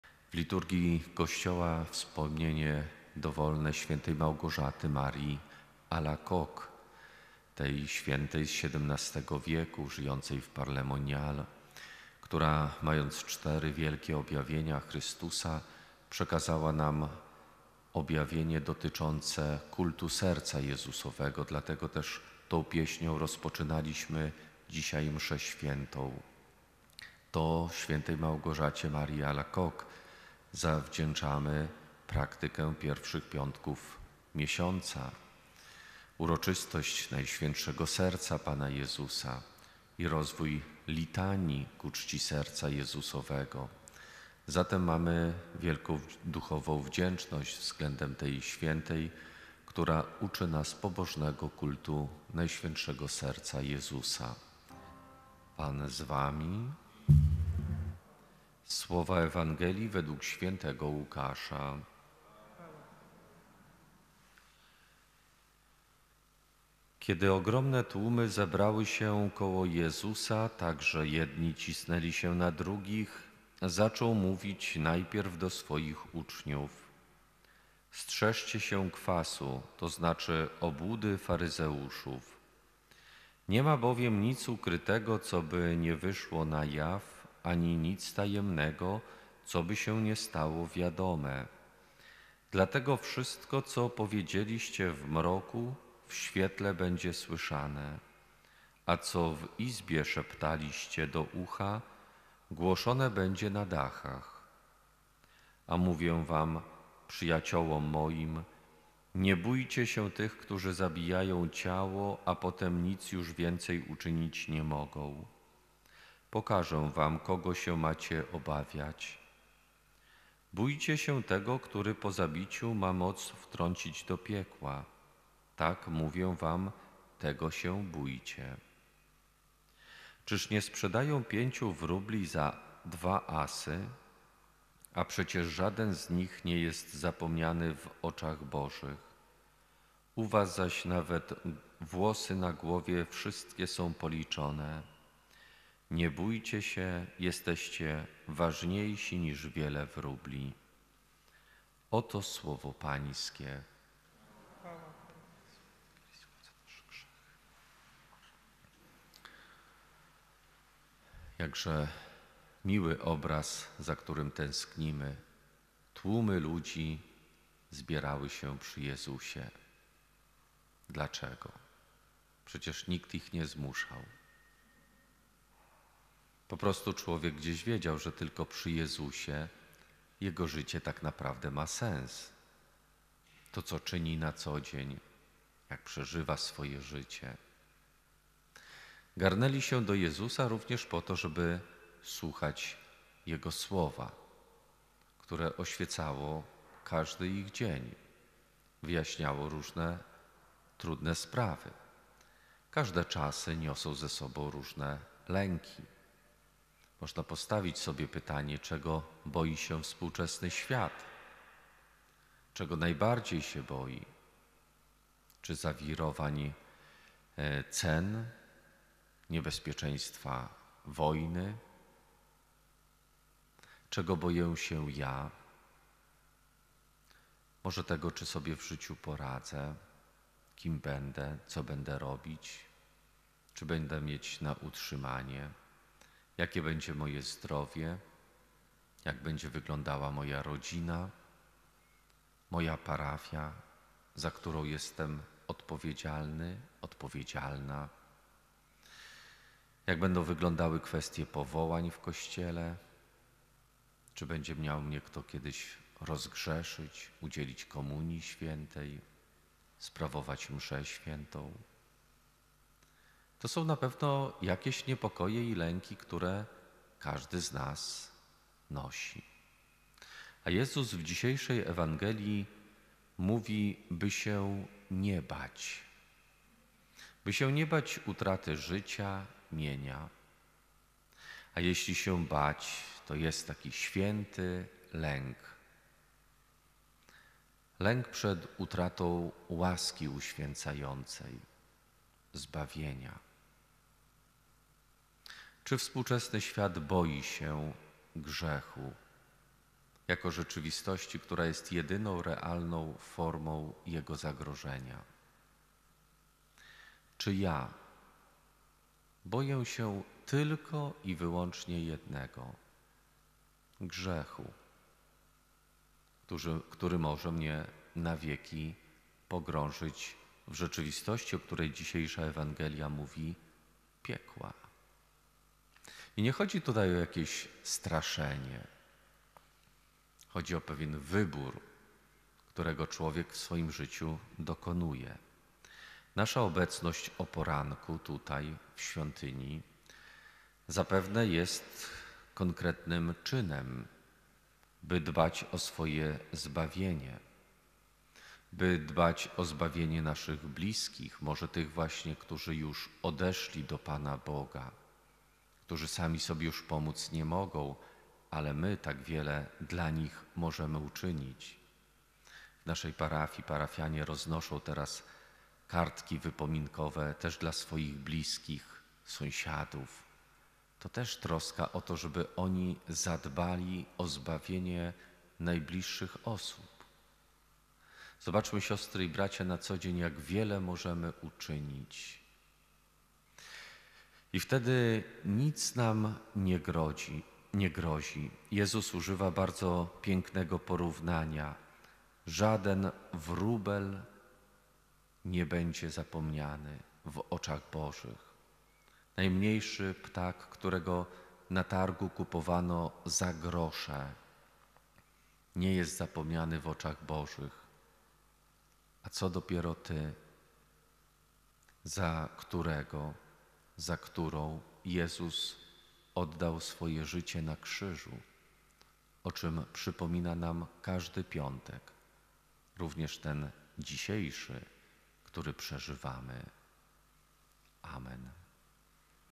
DobreSłowoNaDzieńDobry | Św. Małgorzata Maria Alacoque | 14.10.2022 | Łk 12, 1-7
Codzienny komentarz do czytań mszalnych księży z parafii św. Józefa w Lublinie.

Zapraszamy.